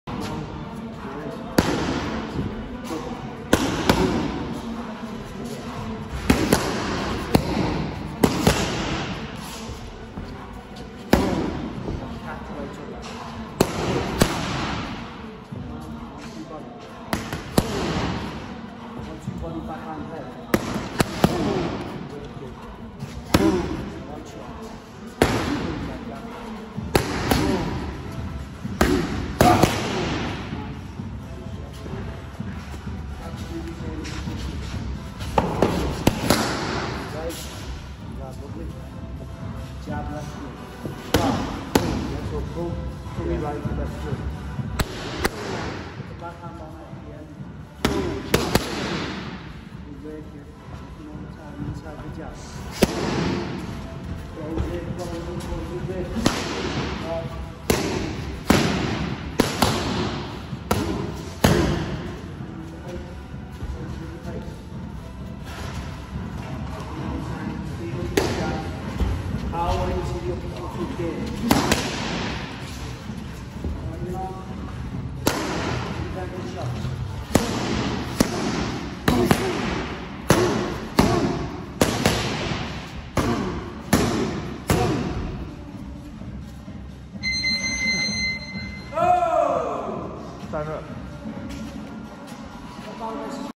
Mike McKay Boxing Pad Work
Former GB boxer and now professional boxer Mike McKay on the pads with coach Rob Butler at Rydal boxing gym Liverpool.